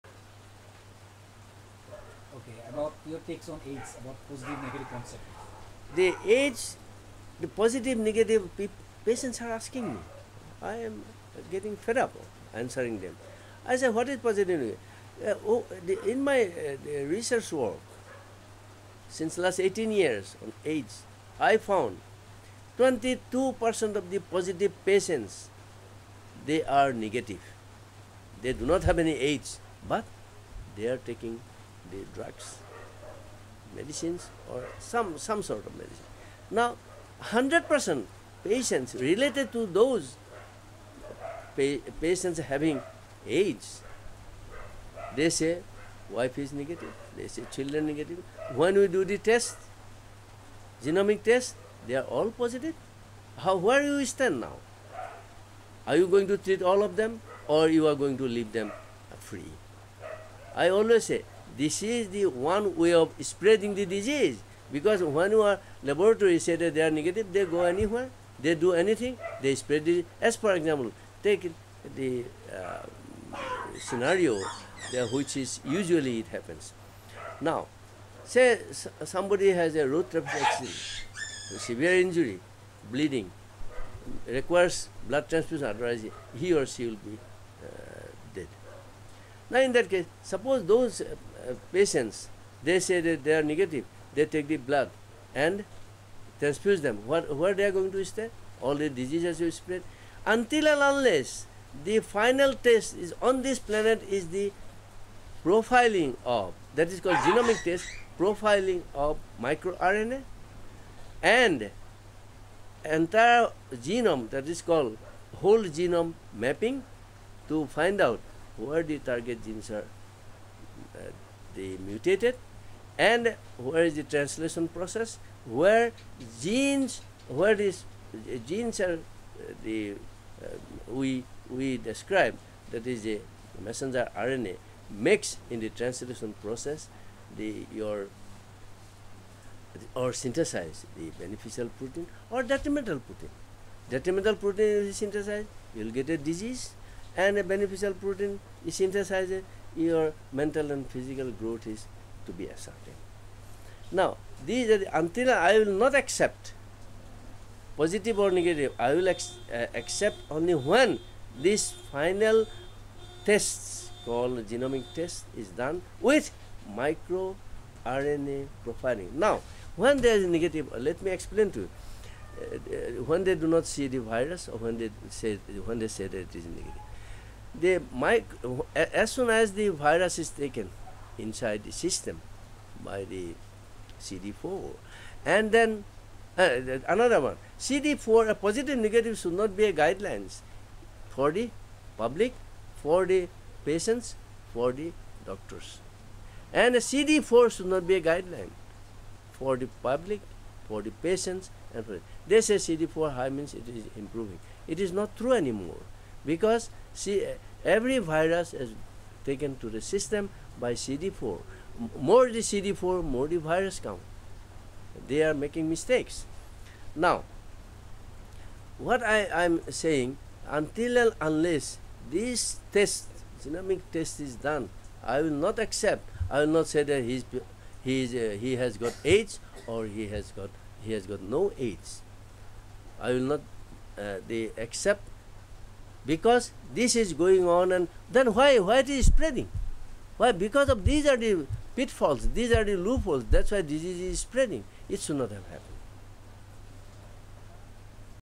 Dr. Dhaniram Baruah, on HIV-AIDS mis conception, raw interview
Dr. Dhaniram Baruah, heart surgeon,(FRCS Glassgow) and a genetic engineer at present working on his own world  of Genetic science in Assam.